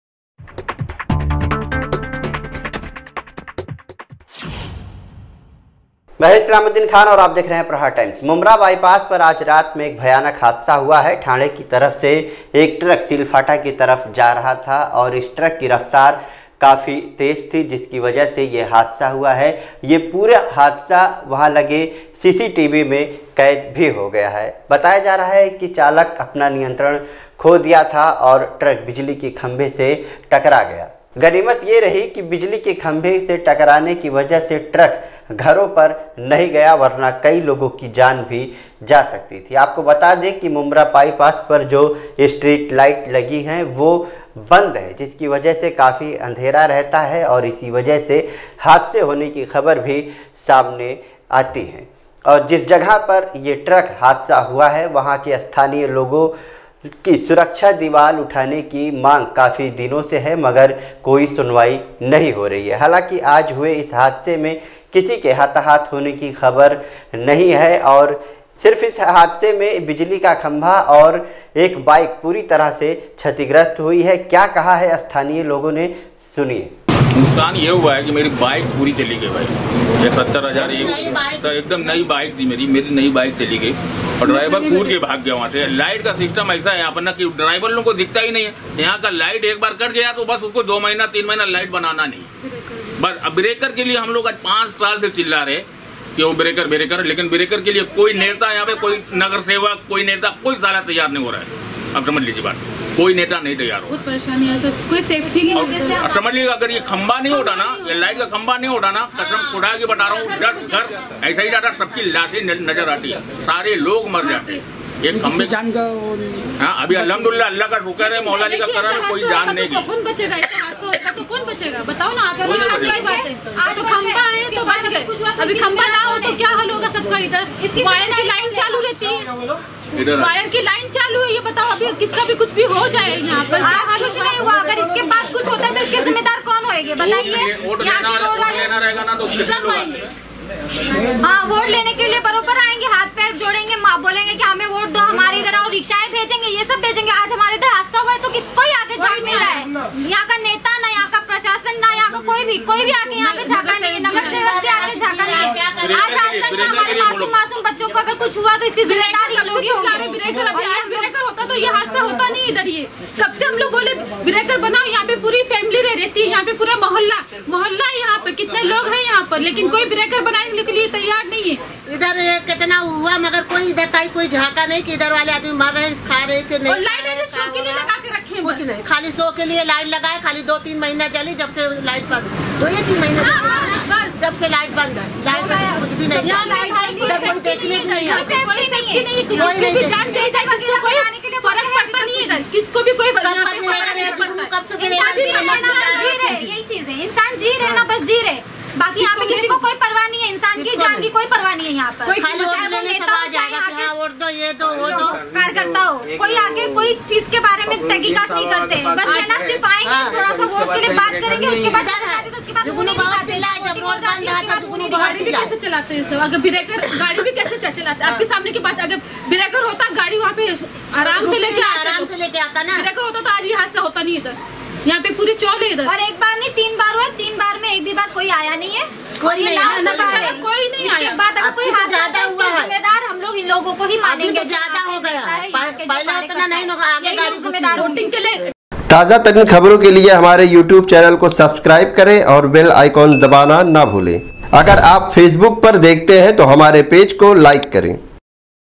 मुम्ब्रा बायपास पर भयानक ट्रक हादसा लोगो में काफी गुस्सा||Aapka Prahar Times
मुम्ब्रा बायपास पर भयानक ट्रक हादसा  लोगो में काफी गुस्सा
बायपास की स्ट्रीट लाइट बंद पड़ी है
इस जगह पर कई बार हो चूका है हादसा
चालक हादसे के बाद हुआ फरार